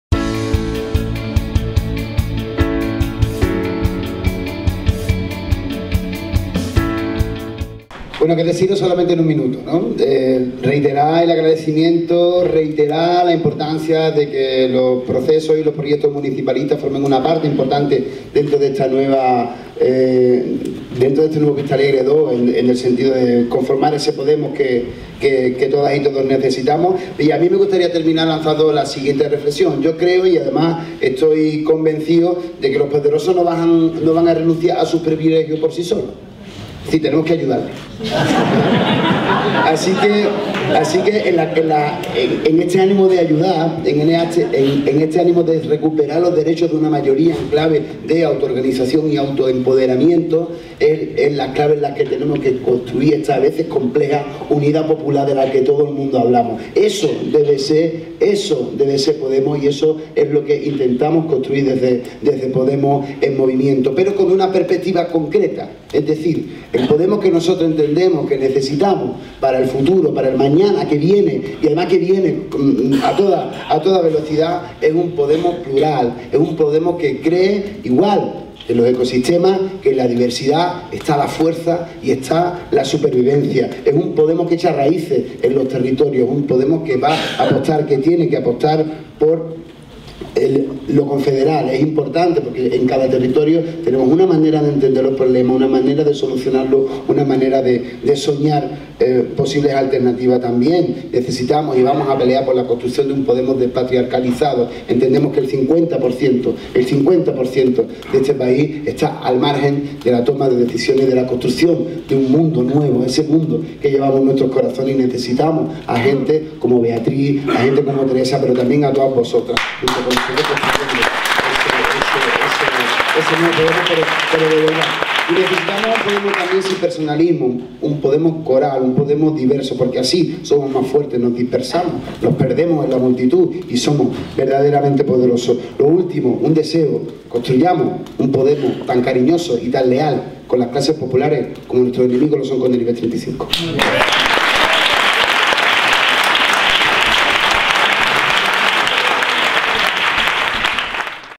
KICHI - Alcalde de Cádiz: Los poderosos no van a renunciar a sus privilegios por sí solos
Kichi en el acto de presentación de Podemos en Movimiento: "El Podemos que necesitamos es un Podemos plural y despatriarcalizado, que eche raíces en los territorios."

El Alcalde de Cádiz, José Mª González “Kichi”, ofrece su punto de vista sobre lo que significa “Podemos en movimiento" y deja algunas frases de interés:

回 Estoy convencido de que los poderosos no van a renunciar a sus privilegios por sí solos, es decir, tenemos que ayudarlos.

回 Recuperar los derechos de una mayoría en clave de autoorganización y de autoempoderamiento, que establece la compleja unidad popular de la que todo el mundo hablamos. Eso debe ser Podemos.

回 Podemos tiene que apostar por lo federal…; vamos a pelear por la construcción de un Podemos del “patriarcalizado”, entendemos que el 50% de este País está al margen de la toma de decisiones”

"Es desde esta perspectiva que nos planteamos los futuros retos de Podemos, y en general del
actual ciclo político. La quiebra del marco cultural y político de las clases medias está detrás de la
crisis del llamado régimen del ‘78. A partir de aquí, sin embargo, las interpretaciones son dispares.
En una parte de Podemos y de los movimientos del cambio (sobre todo en las de matriz “populista”,
cualquiera que sea su definición), existe una lectura no del todo explícita, que señala que es posible
una vuelta a las condiciones precrisis, o al menos, una recuperación del tronco social fundamental
de la democracia española anclado en las clases medias. En esta línea se deben entender las apelaciones
a una masa social moderada y asustadiza, dispuesta a las pequeñas reformas regeneracionistas,
pero no a los cambios profundos, muchos menos a transformaciones sustanciales en la
estructura social y política…” ☛ Para leer el documento completo de “Podemos en movimiento”

Por permitirme la publicación de este vídeo, envío desde aquí mi agradecimiento al canal: Podemos Movimiento

Reconocimiento / Atribución…

NOTA: Vídeo publicado el 13/12/2016